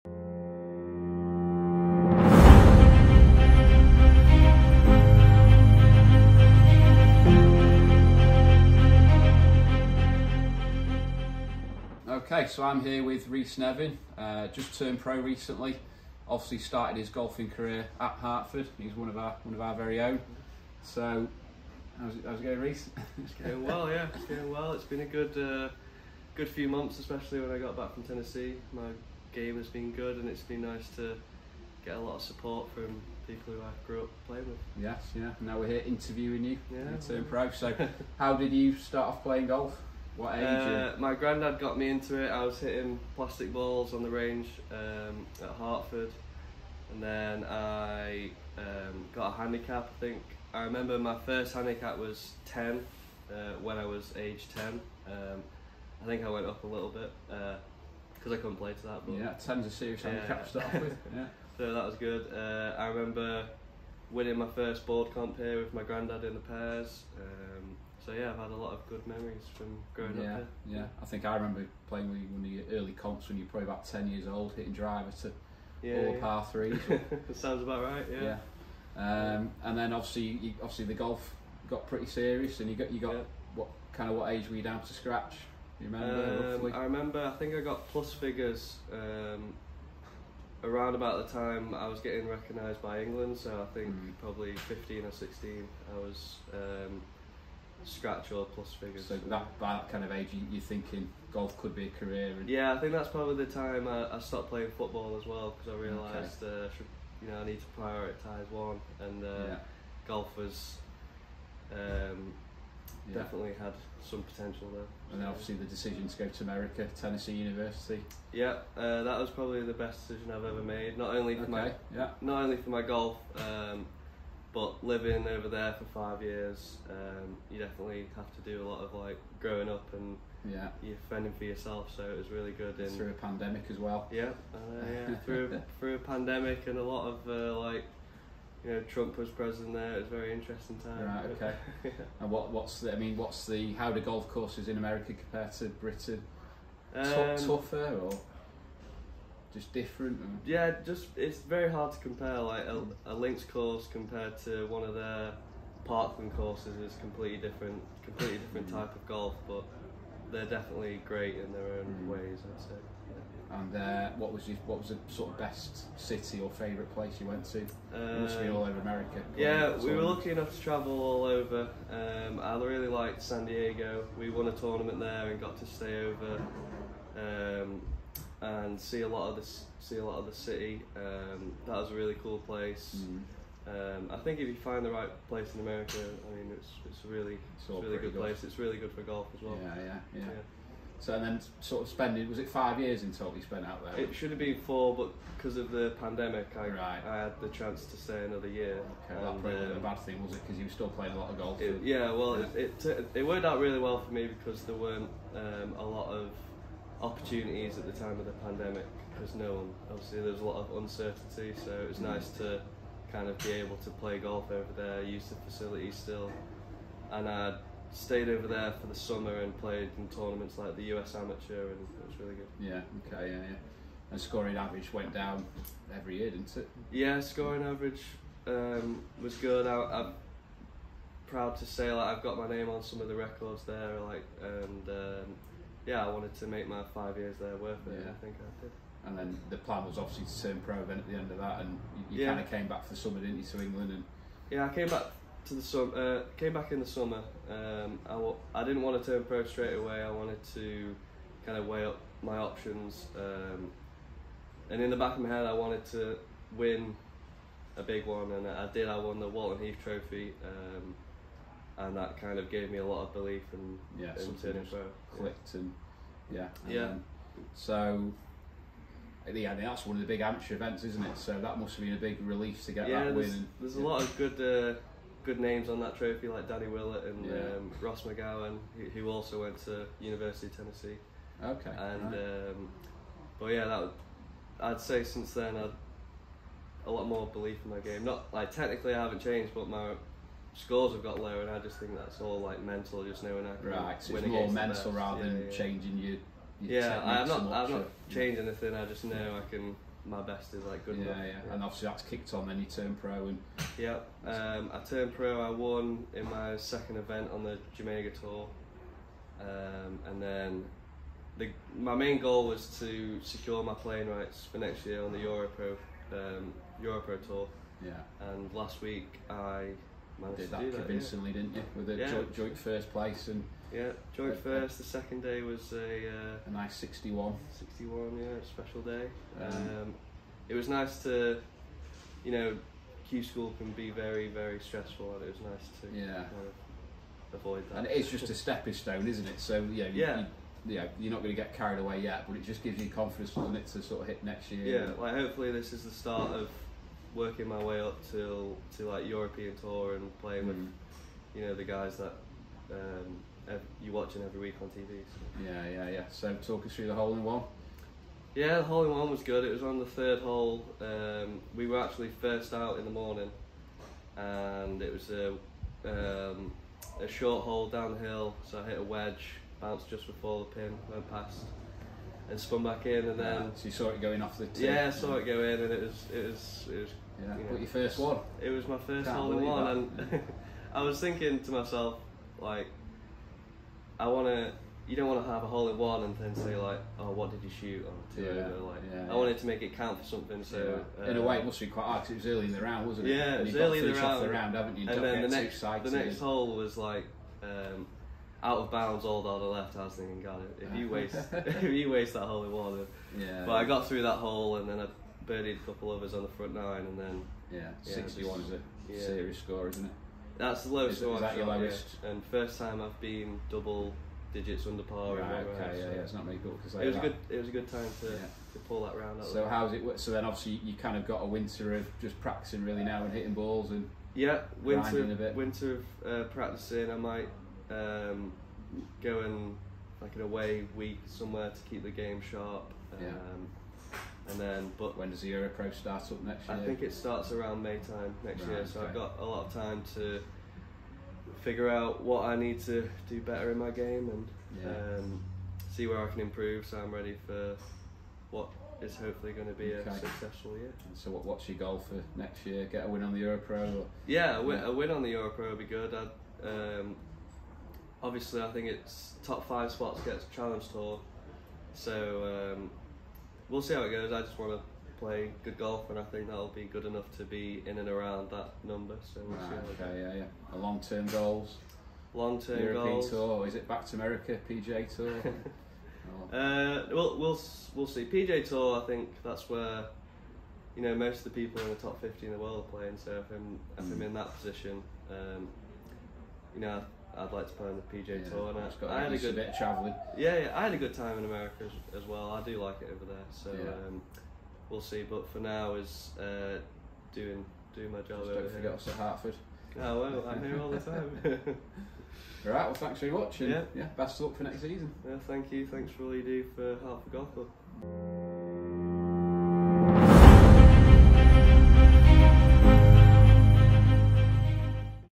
From a 10 handicap to playing professional golf!!
We catch up with our most famous club graduate - Rhys Nevin, he talks us through a successful end to the summer in which he turned professional and claimed a first win, he also tells us about his plans and goals for 2022.